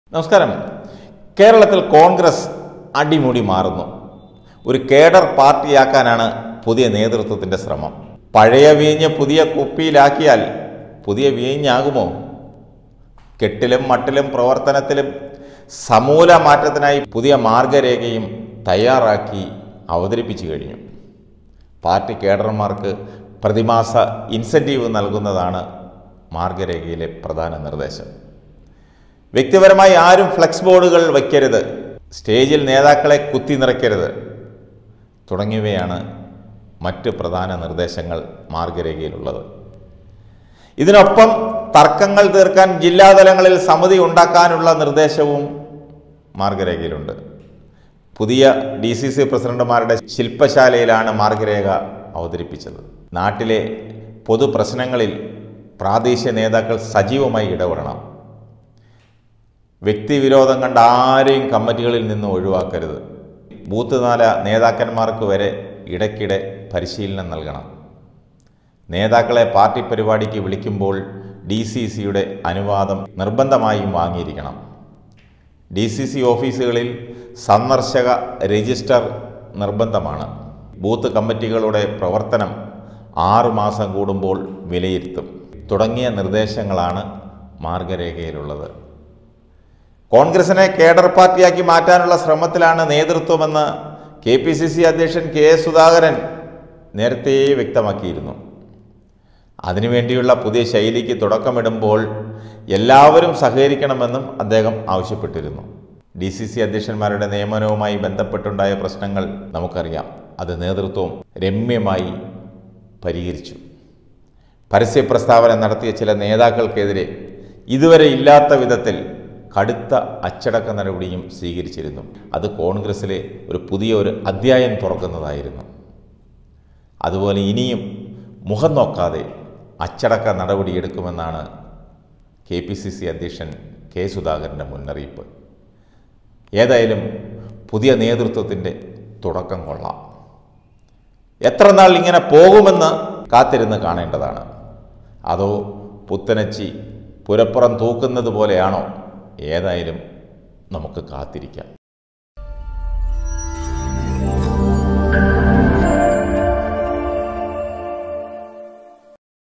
കോൺഗ്രസ്സ് കേഡർ പാർട്ടിയാക്കും : കേഡര്‍മാര്‍ക്ക് പ്രതിമാസ ശമ്പളം|Congress|KPCC|K Sudhakaran
Tag-Generator-Youtube-malayalam news-global